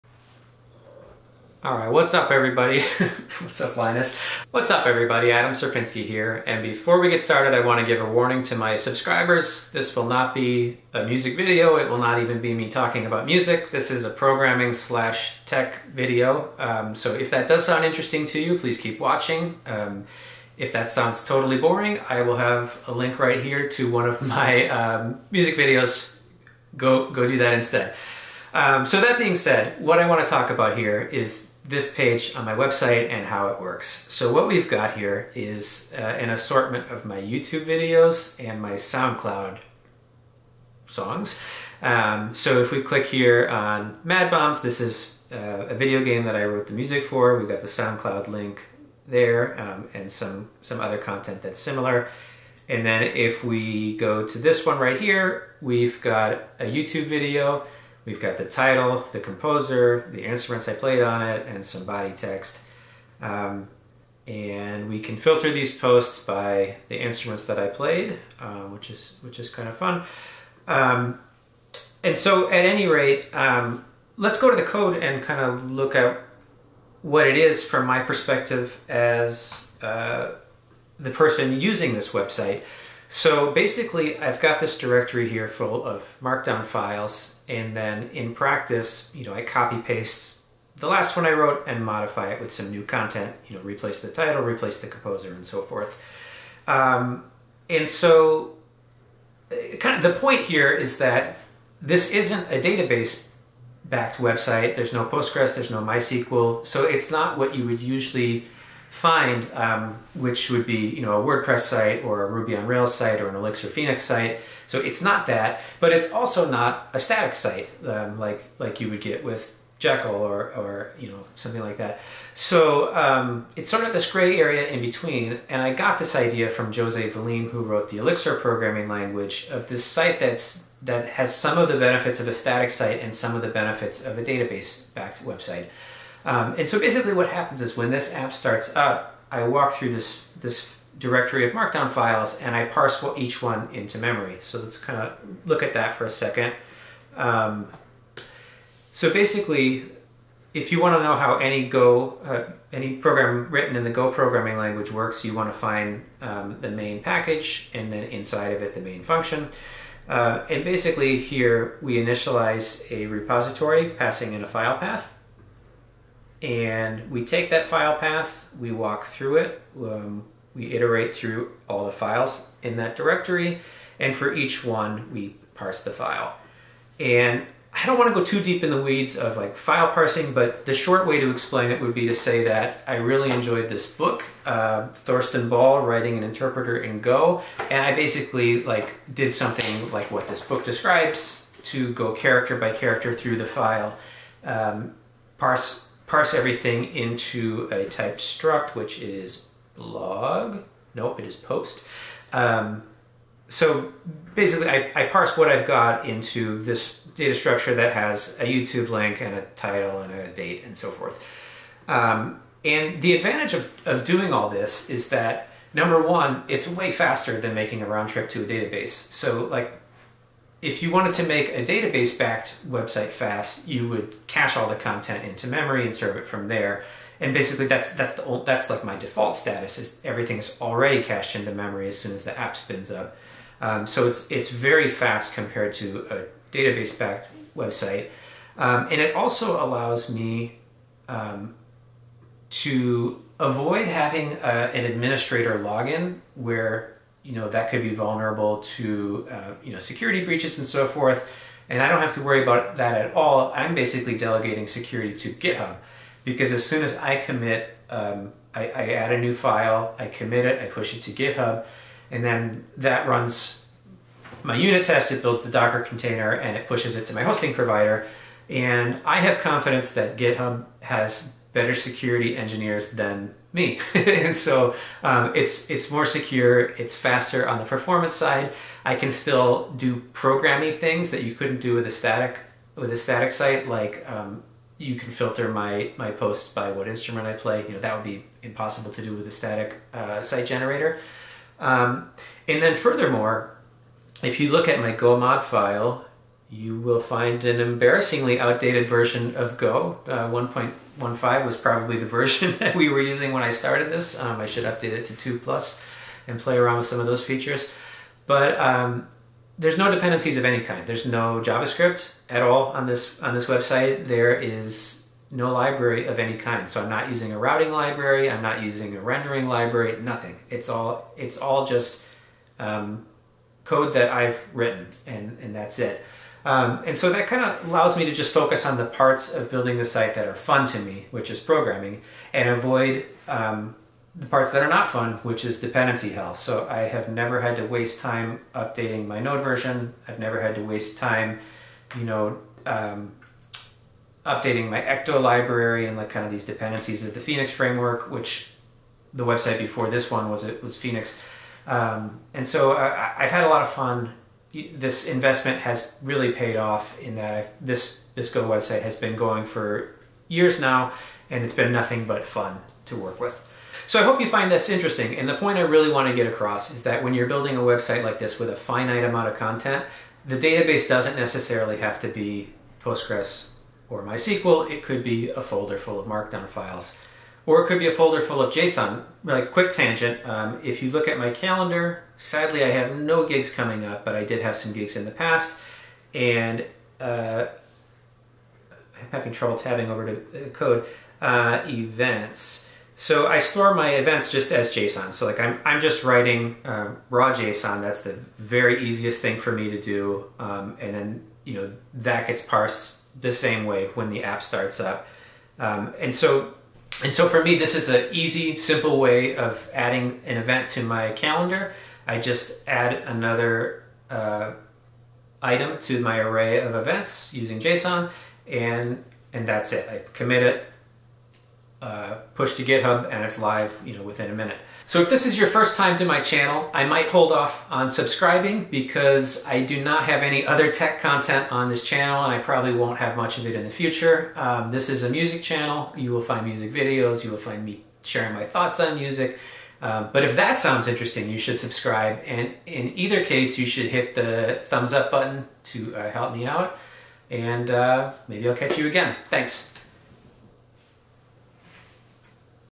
Do you need a database? (how my website works)
Most websites are backed by Postgres or some other SQL database. Mine isn't. Watch the video for the juicy details.